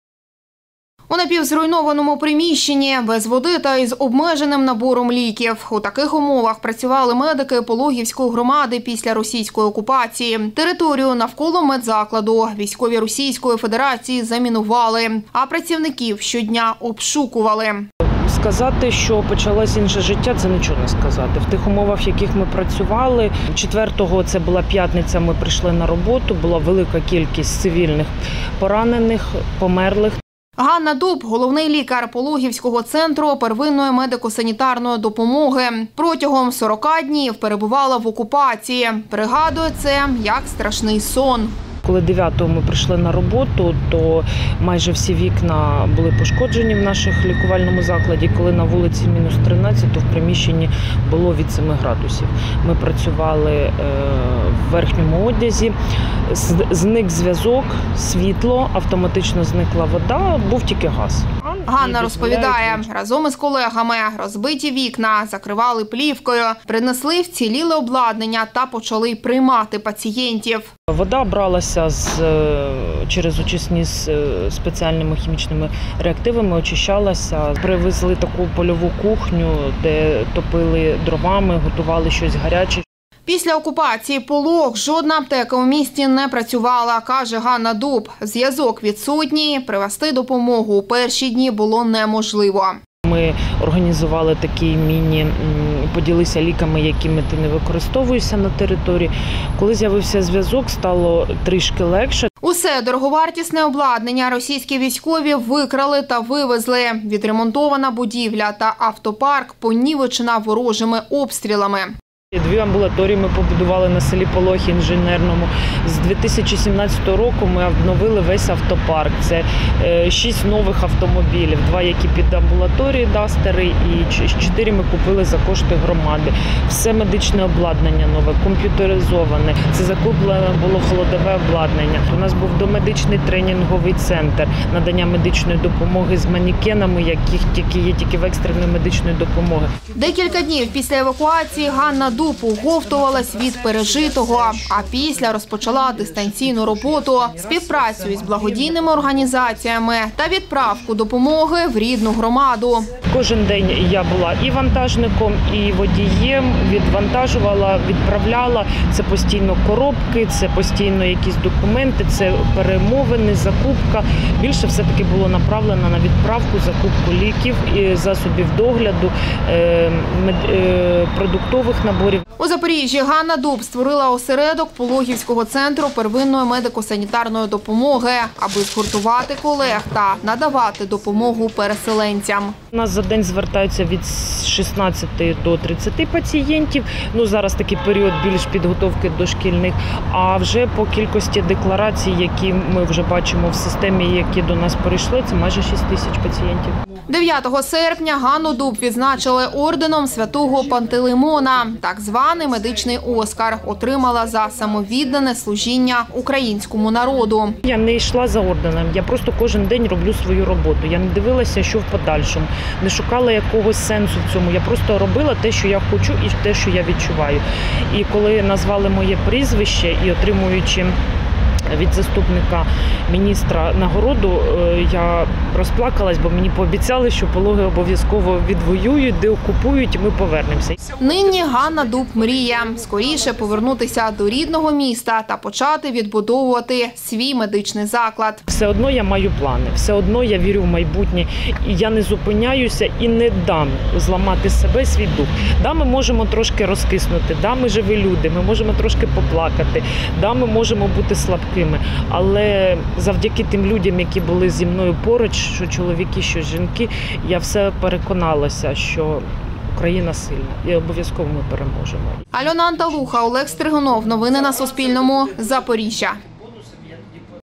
Лікарка Пологівського медцентру про роботу в окупації | Новини
В напівзруйнованому приміщені, без води та з обмеженим набором ліків - в таких умовах працювали медики Пологівської громади після російської окупації. Територію навколо медзакладу військові РФ замінували, а працівників щодня обшукували.